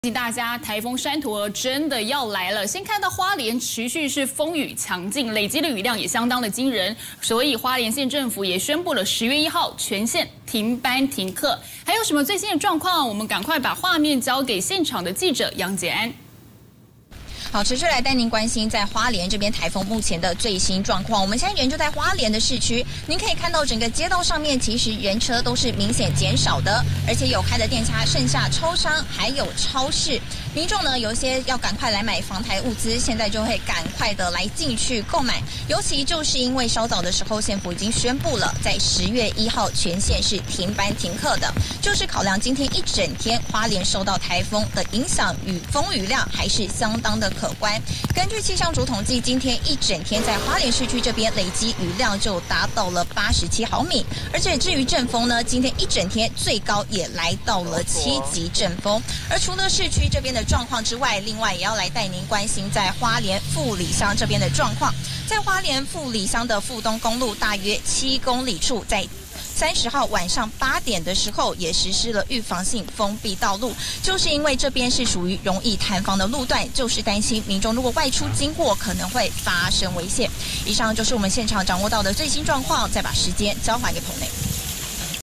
【山陀兒颱風】最新》山陀兒持續增強 花蓮縣10/1停班停課@newsebc
颱風山陀兒在花蓮持續帶來強勁風雨，累積雨量相當驚人，縣府宣布10月1號全縣停班停課。

11/10~11/24 敬請鎖定51頻道！